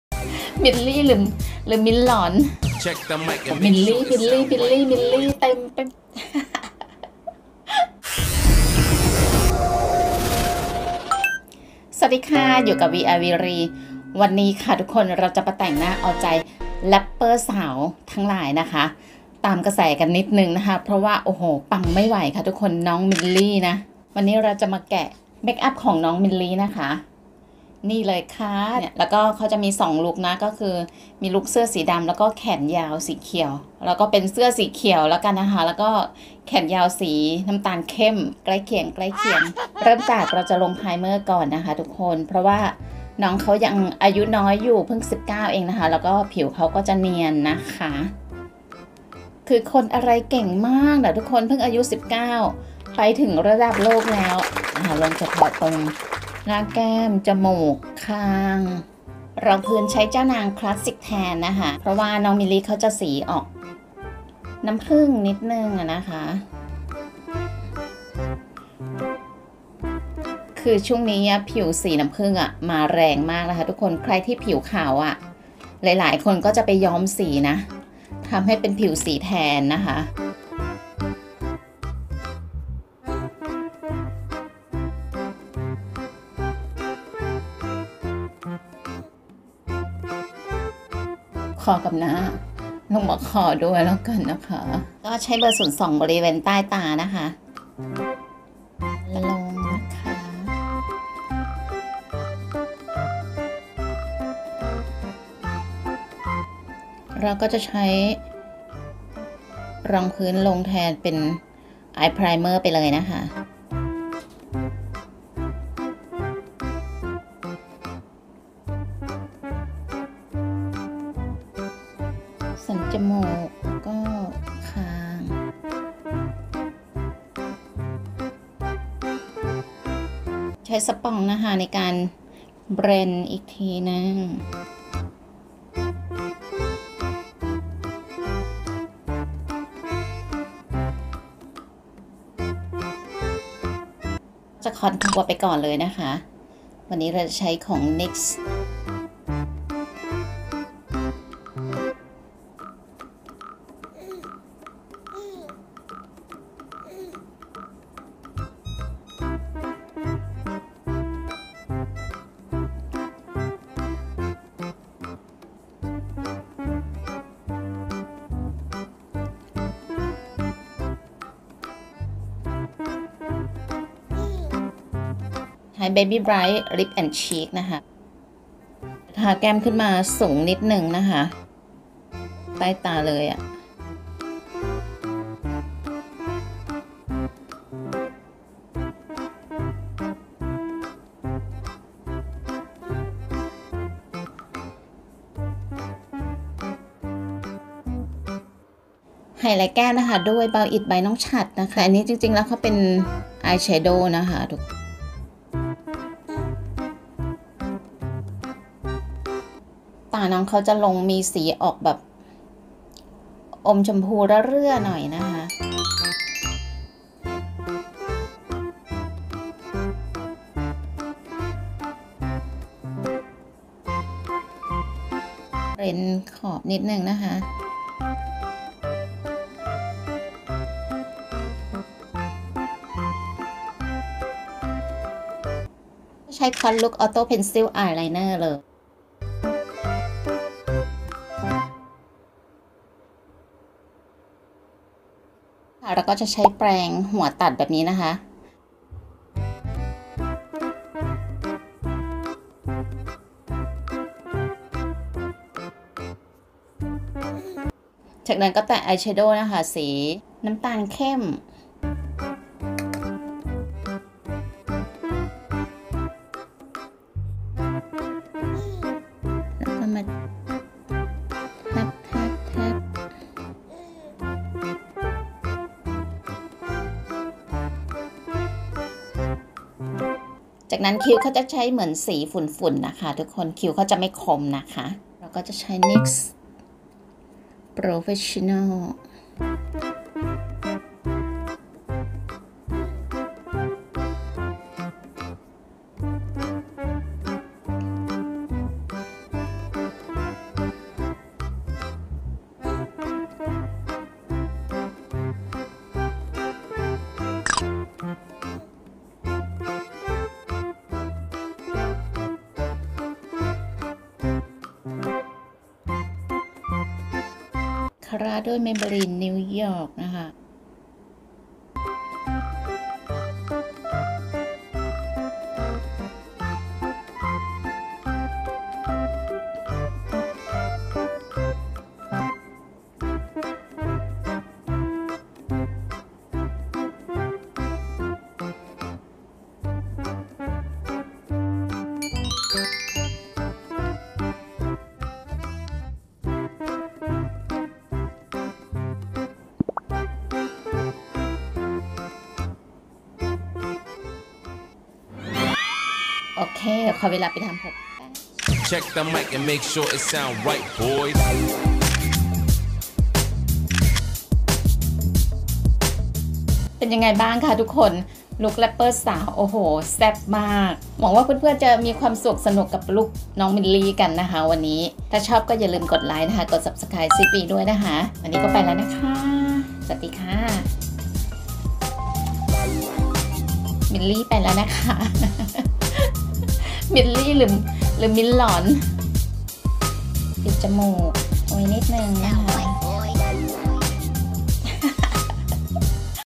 แต่งตามน้อง มิลลิ แกะเมคอัพMilli Rapper ไทยดังไกลทั่วโลก
ชั่วโมงนี้ ปังปุ ไม่ไหว น้องมิลลิ แร็วเปอร์สาวอายุ19
ตังระดับโลก
      วันนี้Sis V ต้องทันกระแส จัดไปค่ะ...
แกะเมคอัพน้องมิลลิ
     จะป้ง ระดับไหนไปดูคลิปกันค่ะ
♥️เมคอัพที่ใช้:
Chaonang Radian Skin #03 Classic Tan

   ติดตามเพิ่มเติมที่นี่นะคะ⤵️